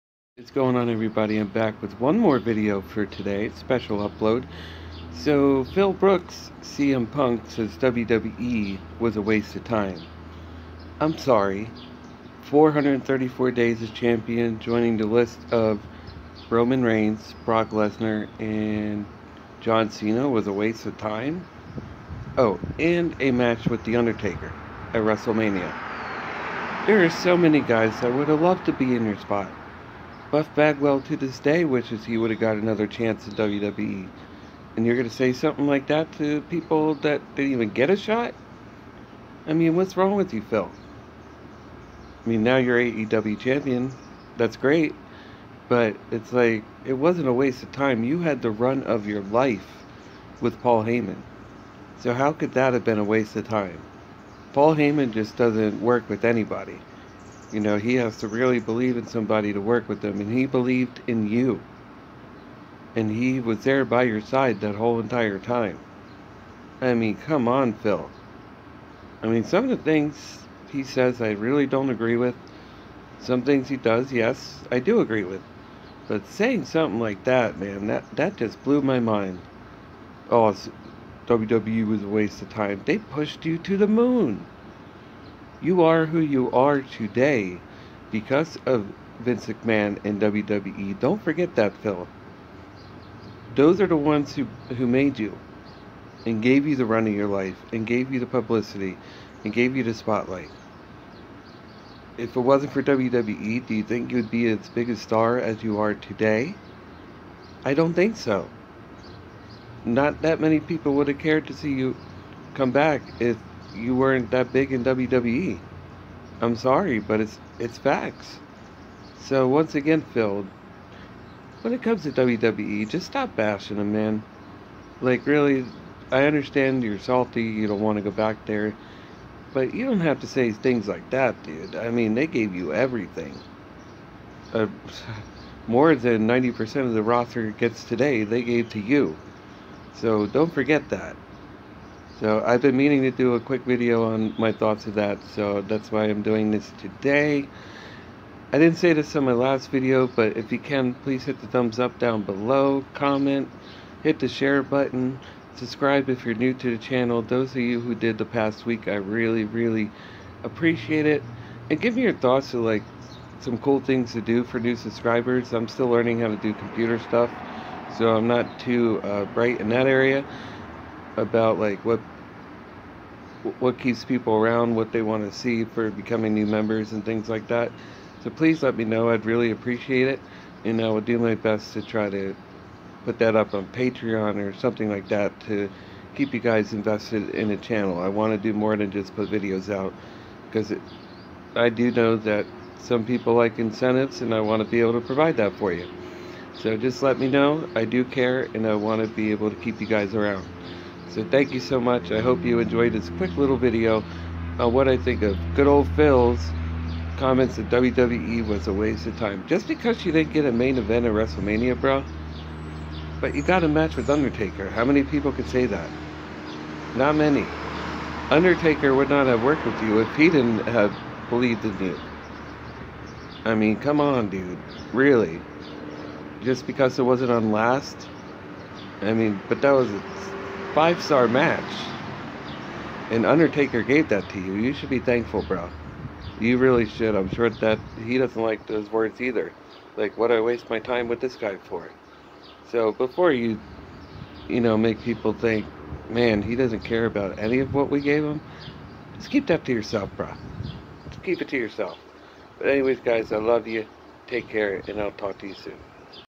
WWE was a waste of time Phil? Guess 434 days means nothing?
I guess 434 days, and a match with Taker means nothing to Phil!